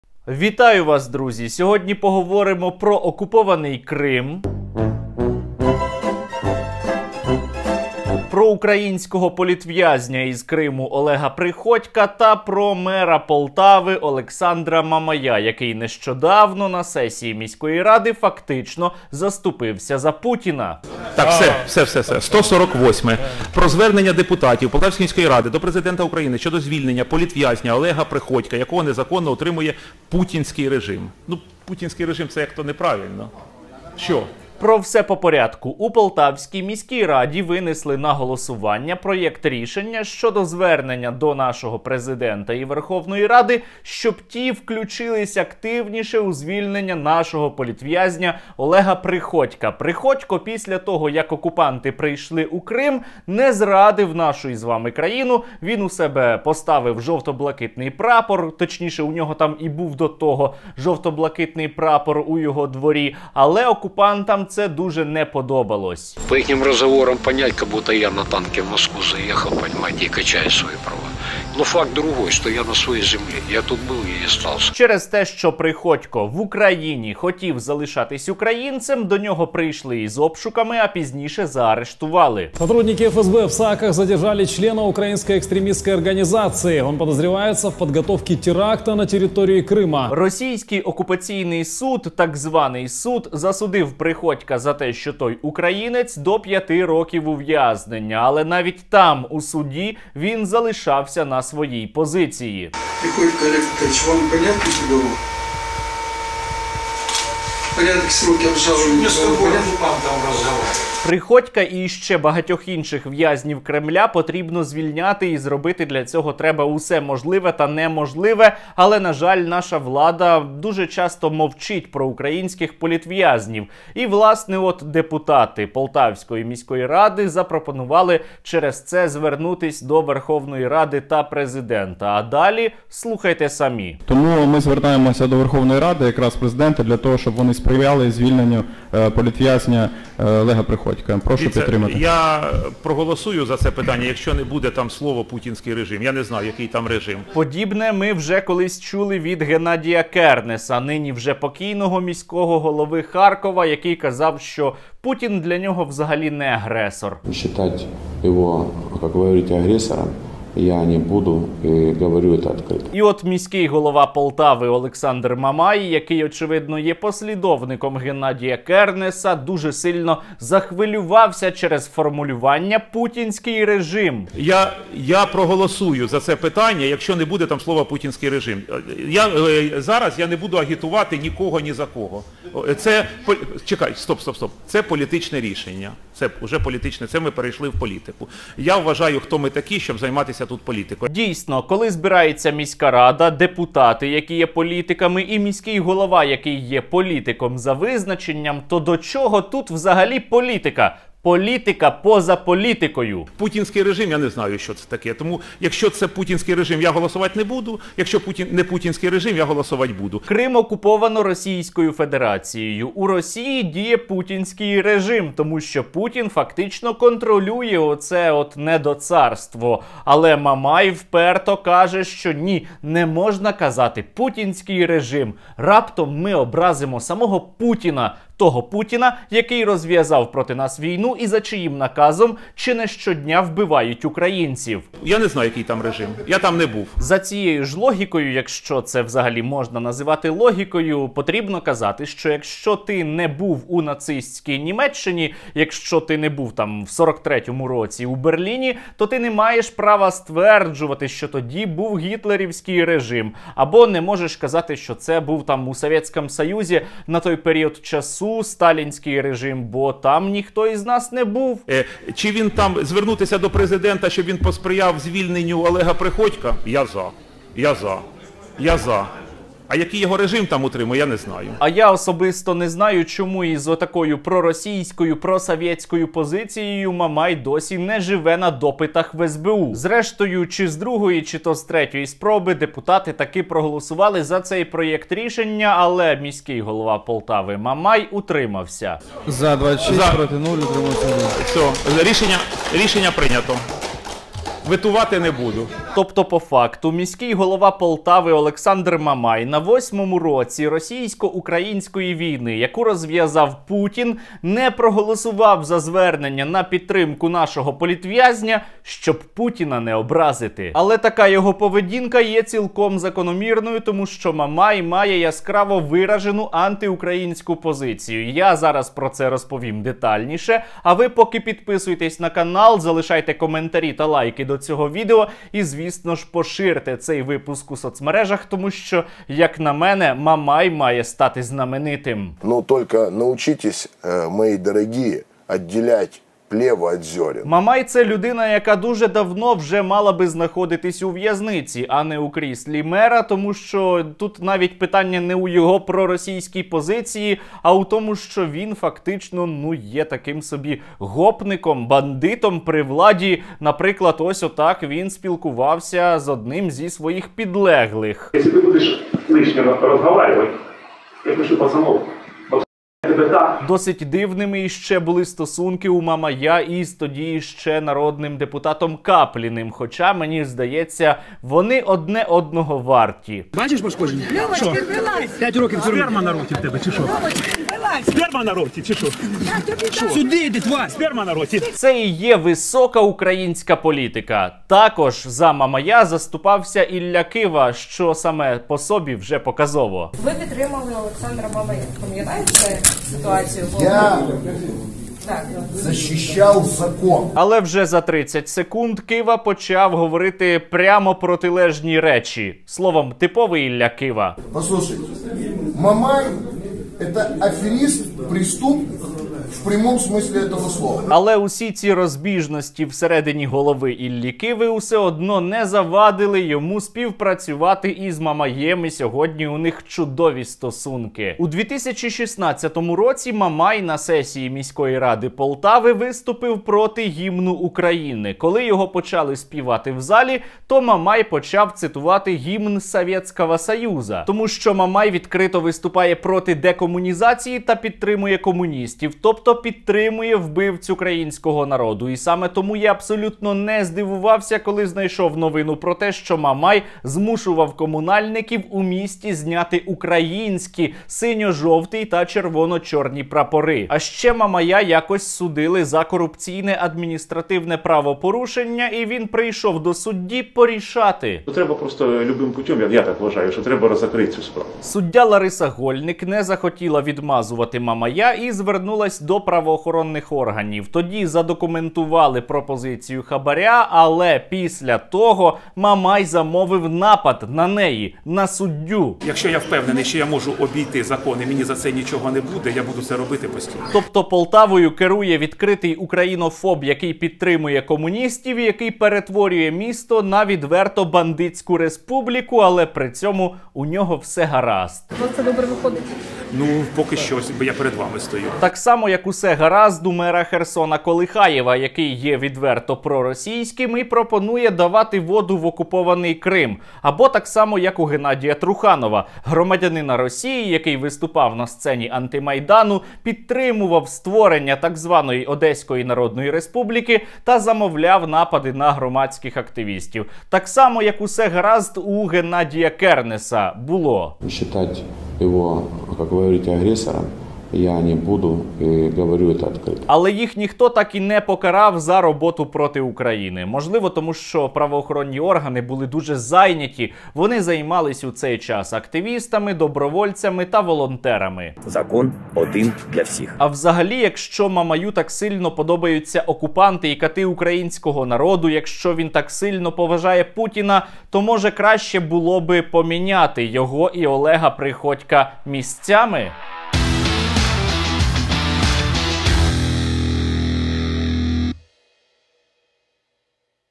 Мер Полтави заступився за путіна та не підтримав кримського політв'язня Приходька
Блог про українську політику та актуальні події в нашій країні "СТЕРНЕНКО НА ЗВ'ЯЗКУ" 🇺🇦